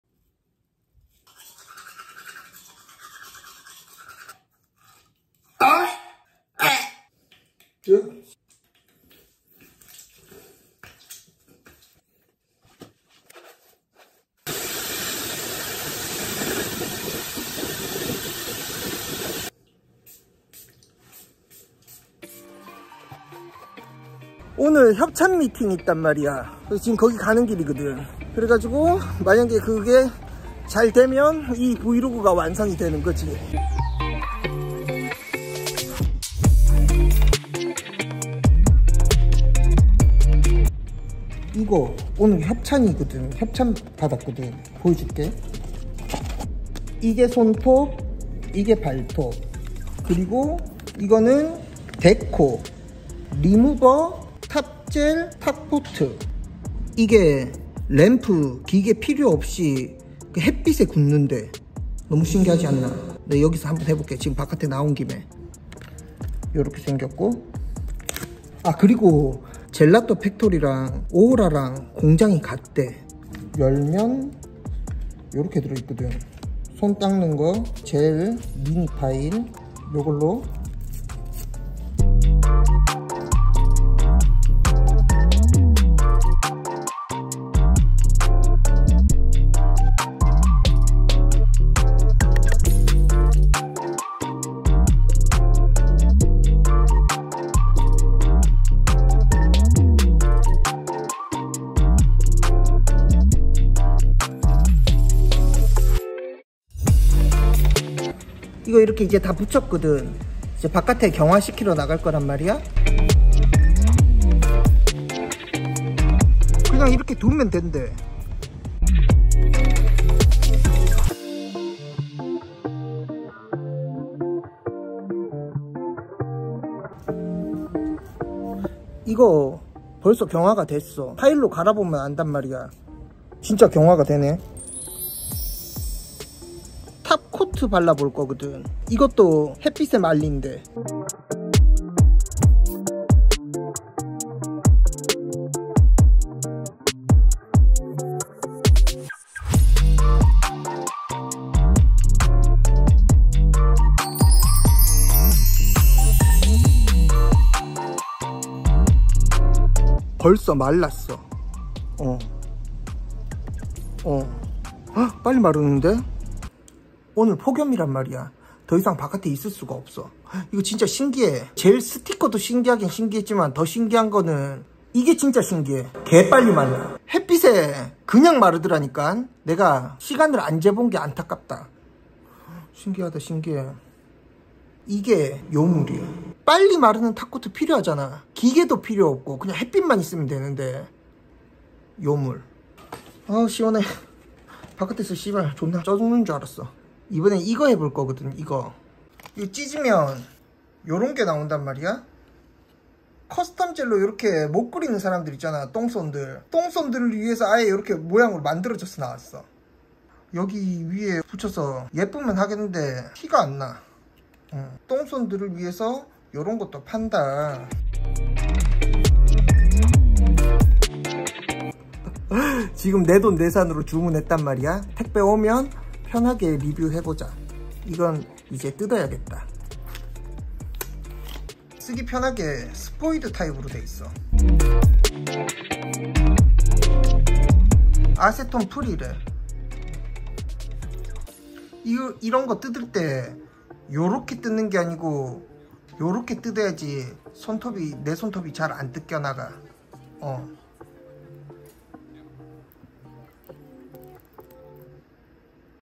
공쥬의 첫 협찬 Vlog (feat.젤라또팩토리)
협찬(단순물품제공) 제의가 와서
브이로그 한번 찍어 봤그든

램프없이 자연광 만으로
젤네일 스트립 경화 시키는것도 신기한데
미리써본 트윙클탑코트
(2023년 7월 24일 판매 예정)
마르는 속도가 더 신기함

첫 협찬 영상이었는데
좋은 경험 만들어준
젤라또팩토리 관계자분
감사합니day
건승하세요^^